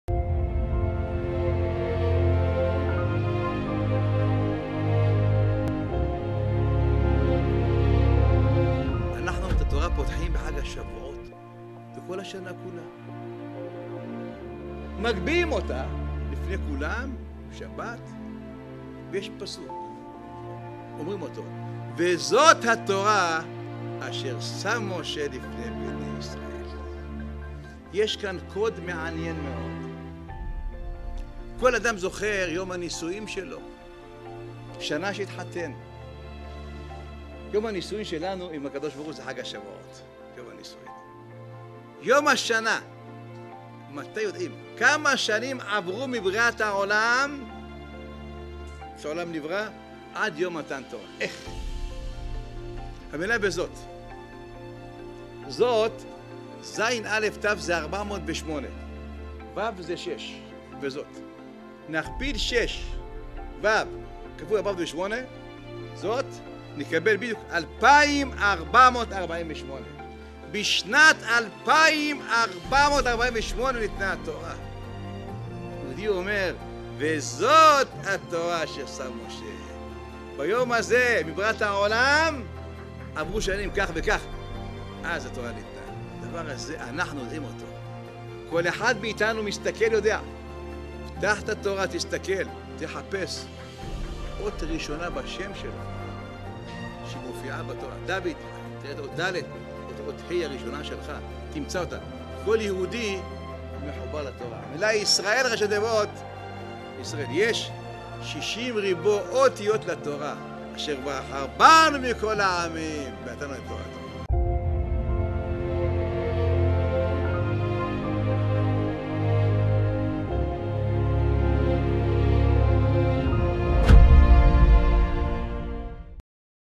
מוסר ברגע: תאריך החתונה הגדולה - הרב הרצל חודר HD
הרב הרצל חודר בפינה יחודית מעביר מסר מיוחד בדקה וקצת והפעם: החתונה הגדולה, מהיכן זוכרים את התאריך המדוייק?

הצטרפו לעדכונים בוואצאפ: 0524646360